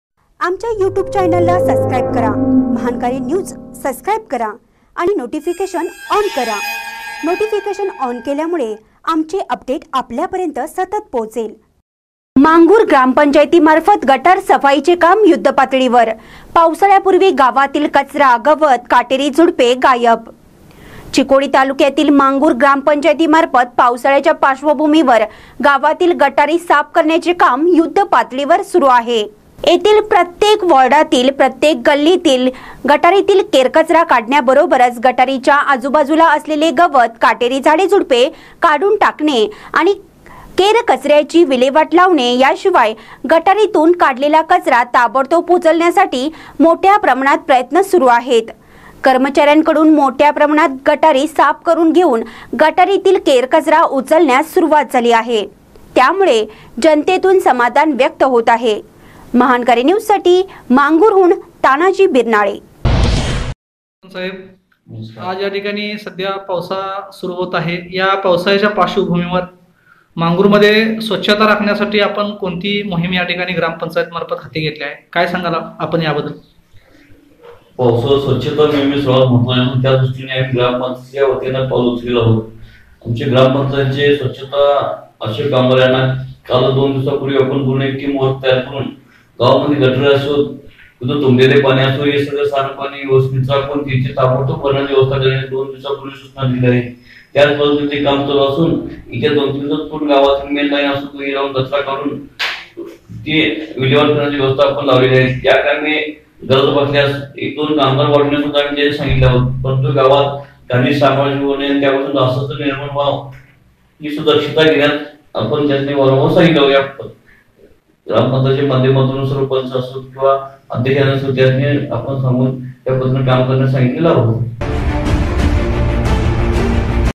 मांगूर ग्रामपंचायतीमार्फत गटार सफाईचे काम युद्धपातळीवर
मांगूर ग्रामपंचायतीमार्फत गटार सफाईचे काम युद्धपातळीवर : पावसाळ्यापूर्वी गावातील कचरा, गवत, काटेरी झाडे गायब